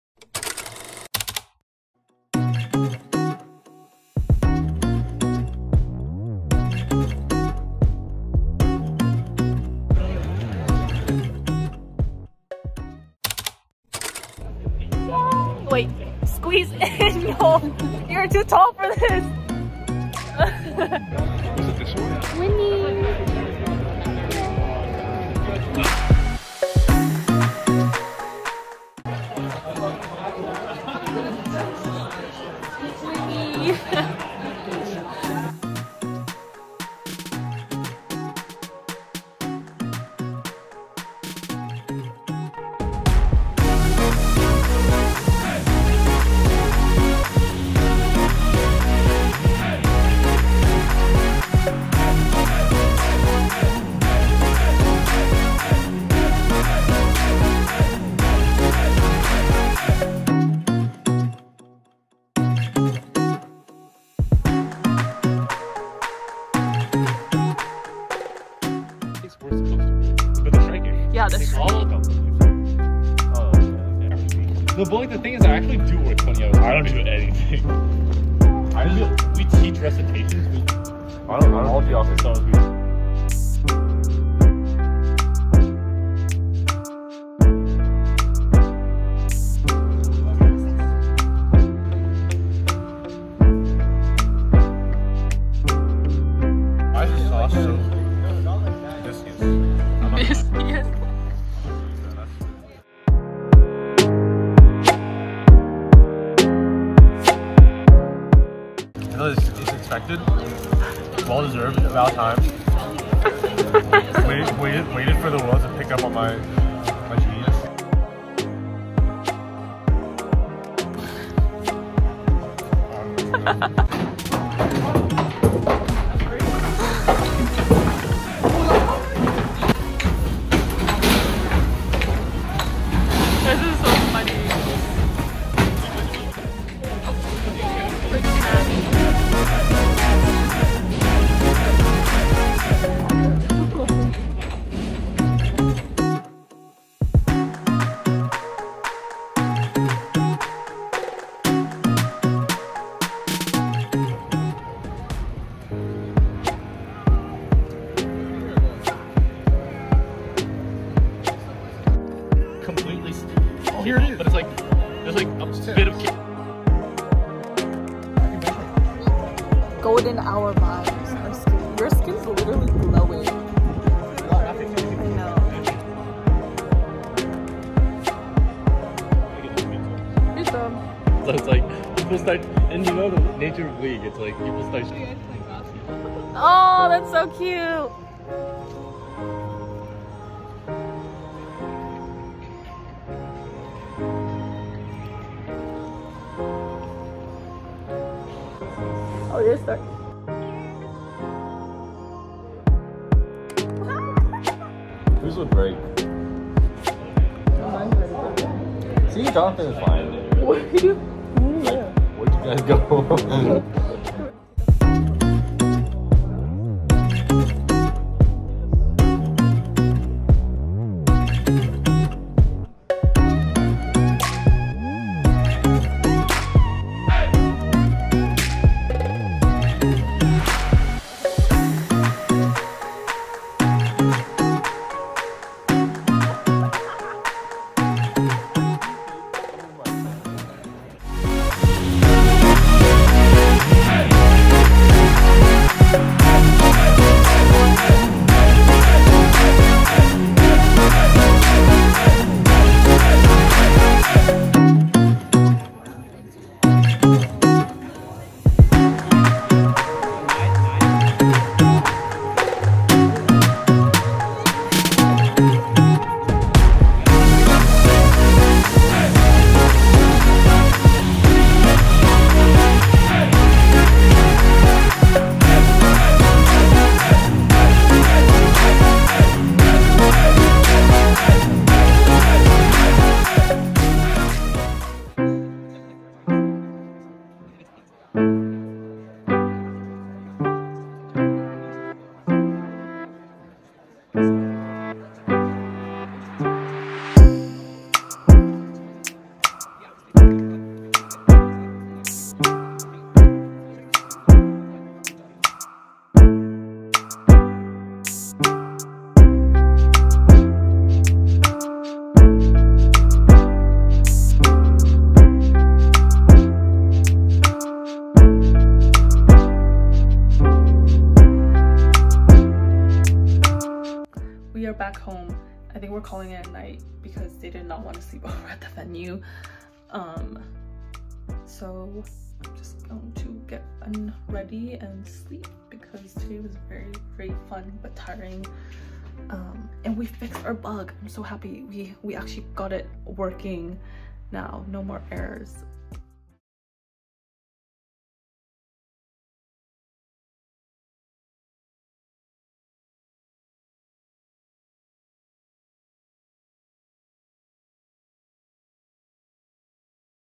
uc berkeley uni vlog 🧸 | i spent 36 hours coding in the world's largest collegiate ai hackathon
hello friends!

Follow me along where I go to UC Berkeley's 36-hour AI LLM hackathon, CalHacks over the summer with over 1300+ hackers! We got access to GPT-4 :0

reminder that you are beautiful & have a great day!! 💛